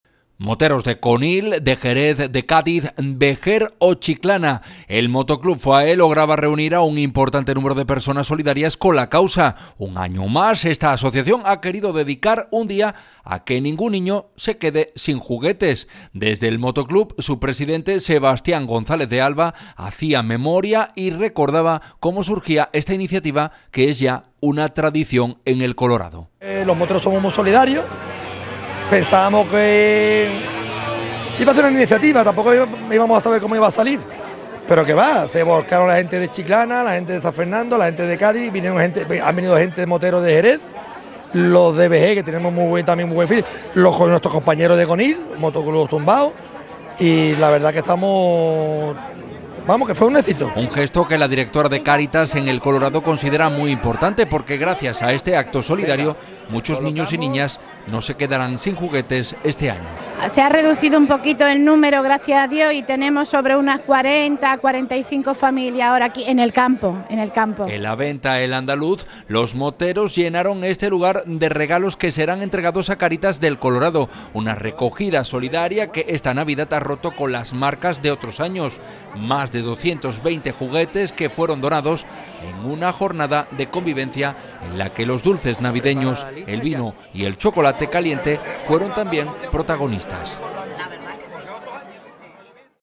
8NOTICIAS JANDA - 10DIC2018
El Motoclub Fuaé logra recoger más de 220 juguetes para los niños y niñas del Colorado. Los juguetes han sido entregados a Cáritas para la campaña de Navidad.